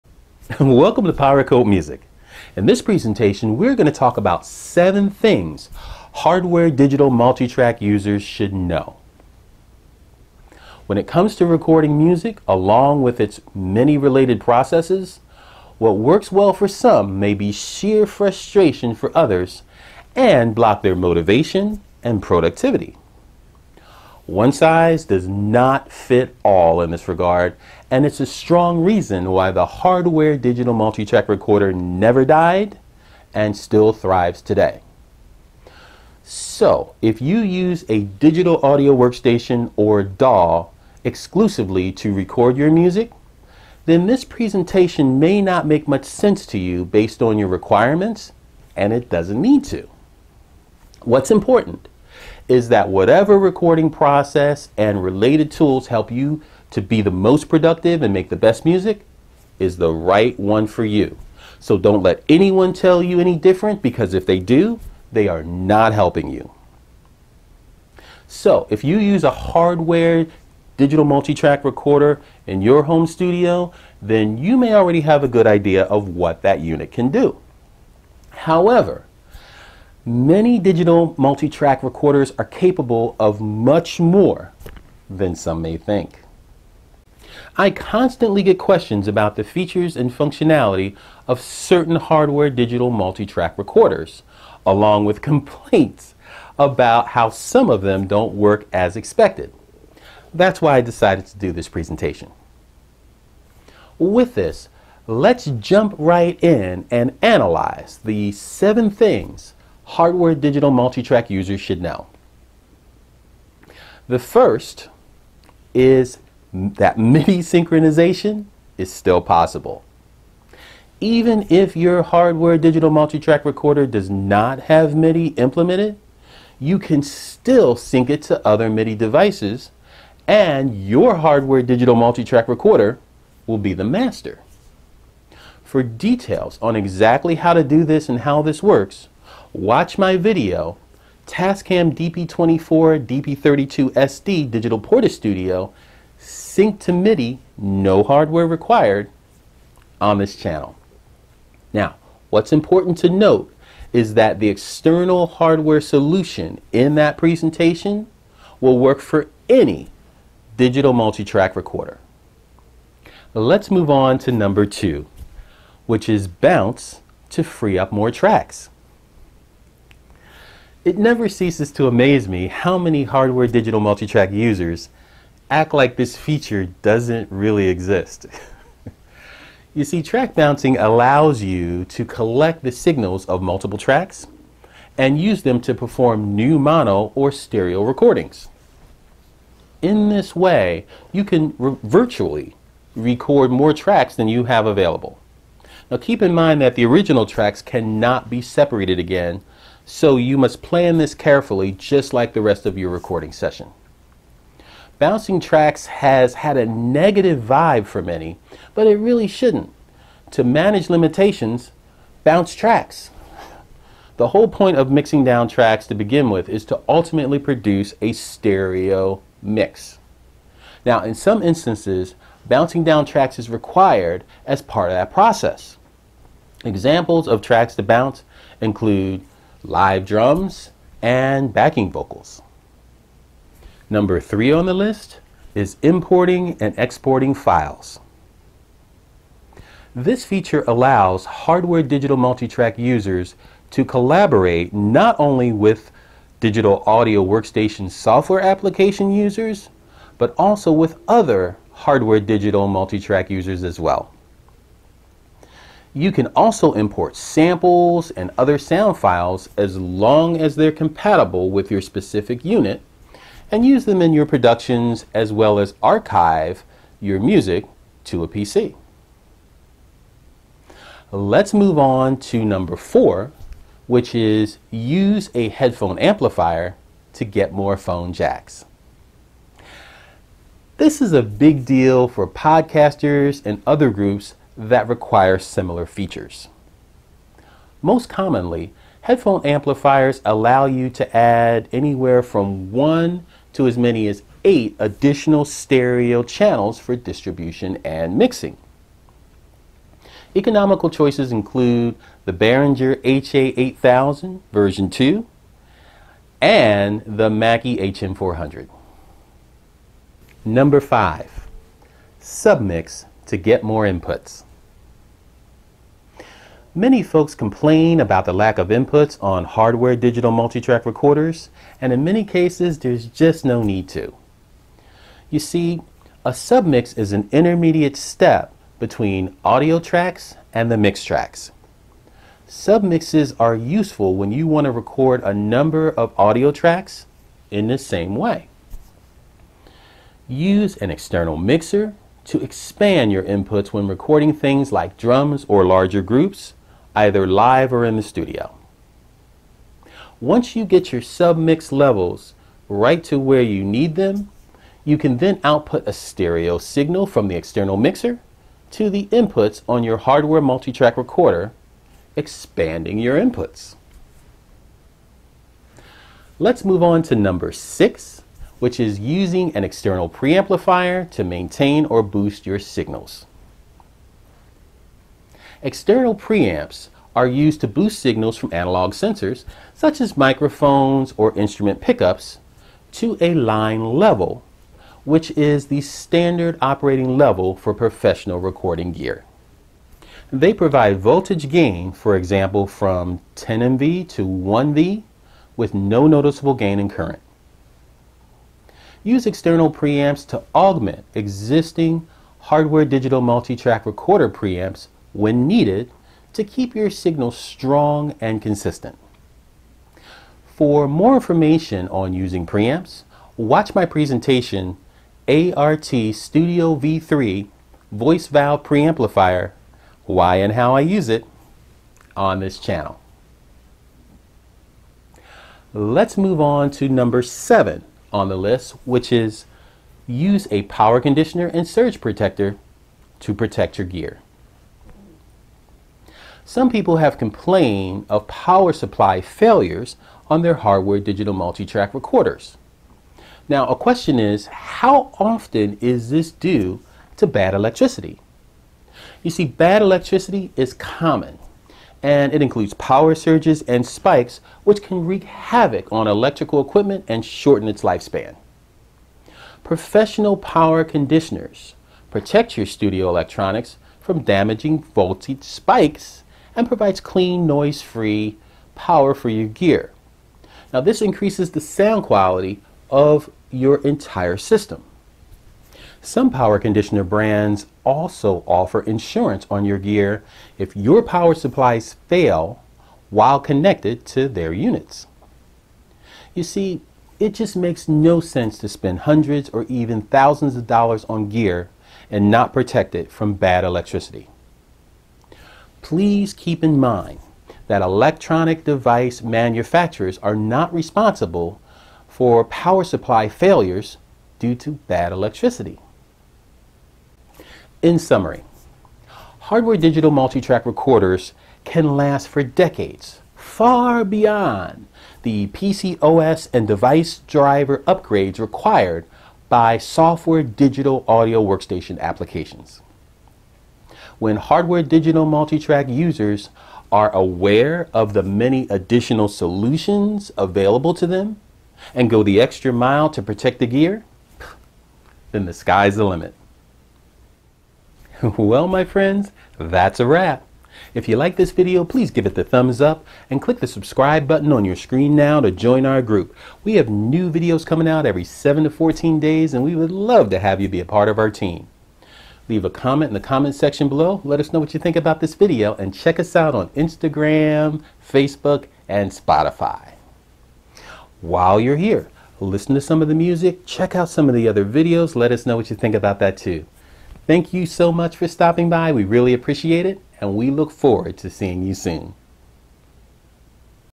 7 Things Hardware Digital Multitrack Users Should Know
Misconceptions abound regarding what some believe are hardware multitrack limitations. The more you know, the easier it is to navigate perceived challenges. Watch all of this presentation now to learn and or reinforce “7 Things Hardware Digital Multitrack Users Should Know” in your home recording studio sessions.

0:00 Introduction
0:07 7 THINGS Hardware Digital Multitrack Users Should Know
1:58 MIDI Synchronization is possible
3:57 IMPORTING & EXPORTING FILES
4:36 Use a Headphone Amplifier for more Phone Jacks
5:14 SUB MIX to get More Inputs
6:11 Use an External Preamplifier to Boost your Signals
7:11 Use a Power Conditioner